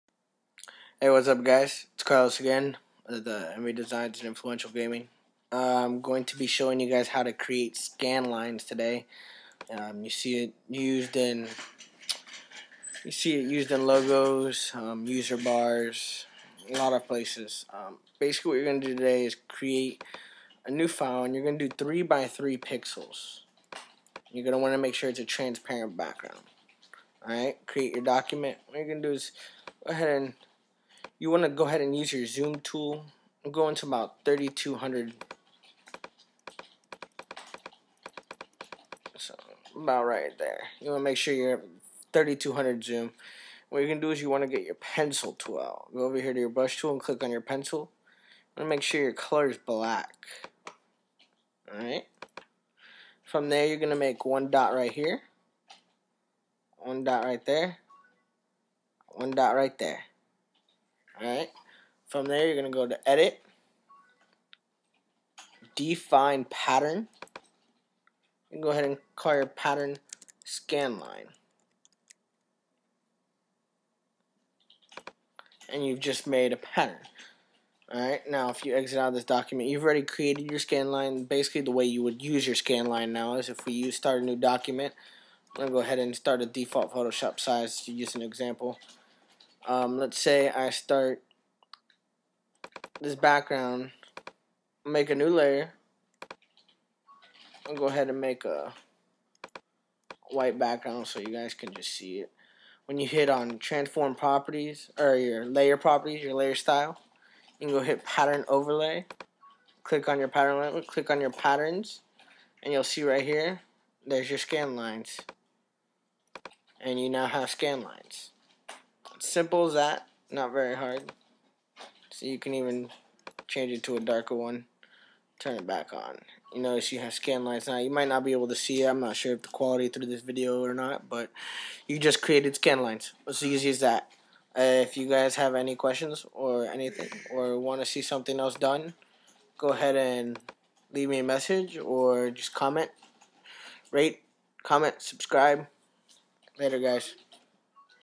How to Create Scanlines
This tutorial shows you how too create scan line pattern in photoshop